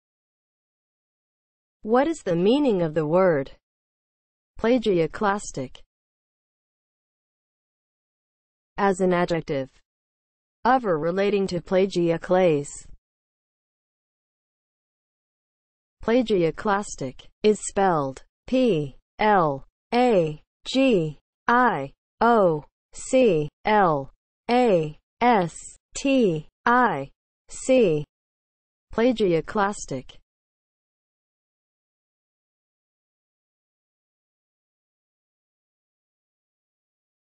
What is the meaning of the word PLAGIOCLASTIC?
Definition and spelling of the word PLAGIOCLASTIC.

(00:00) Intro
(00:07) As an adjective
(00:14) Spelling

As an adjective (00:07):
• of or relating to plagioclase